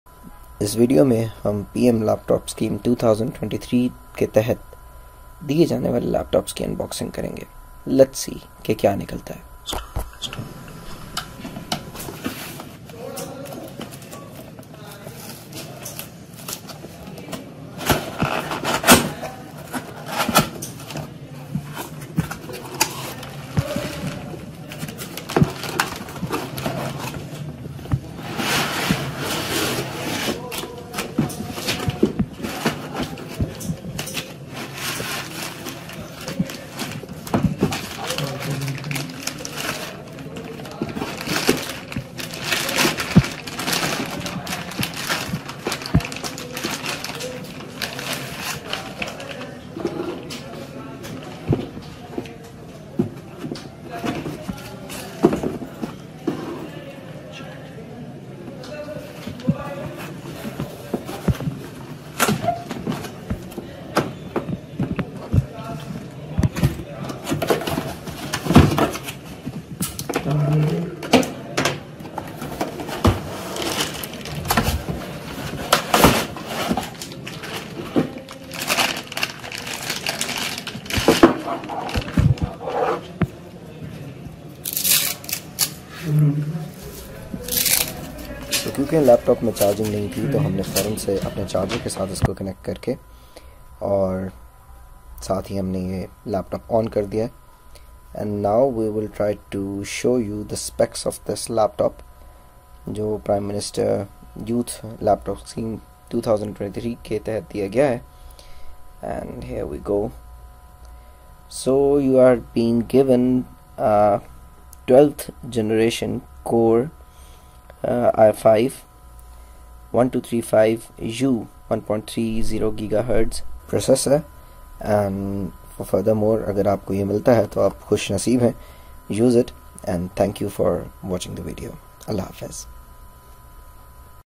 Unboxing PM Laptop Scheme 2023 Laptop
This laptop was received under PM Laptop Scheme 2023 by students of a university, it includes the specs details and completer unboxing of the laptop.